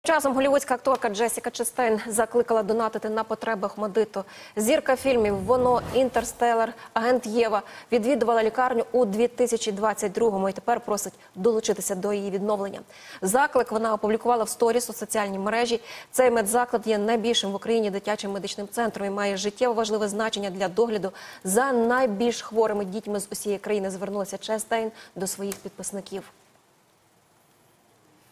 Джессіка Честейн згадала свій візит до "Охматдиту" і звернулася до фанатів із важливим проханням
Зірка Голлівуду підтримала українців після жахливих обстрілів.